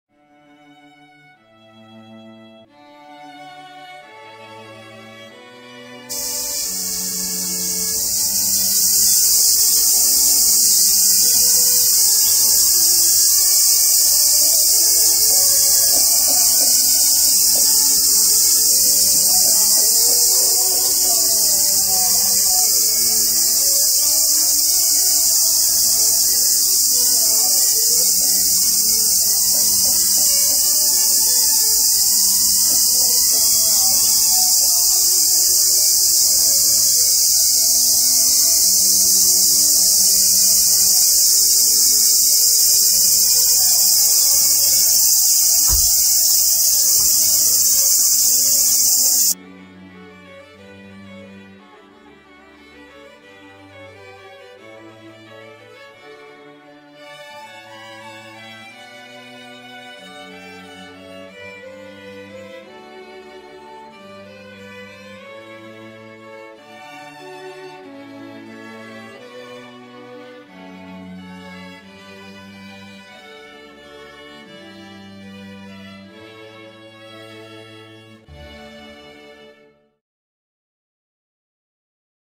Cicada of Korea (Graptopsaltria nigrofuscata) ; Family Cicadidae ; Insect
Cicada of Korea (Graptopsaltria nigrofuscata (Motschulsky, 1866))

Related to the video ; Dry specimen of Cicada

Category : Order Hemiptera, Infraorder Cicadomorpha, Family Cicadidae
Scientific name : Graptopsaltria nigrofuscata (Motschulsky, 1866)
Collection point : South Korea(Jeolabukdo)
Collection date :  26~30. VII. 2021.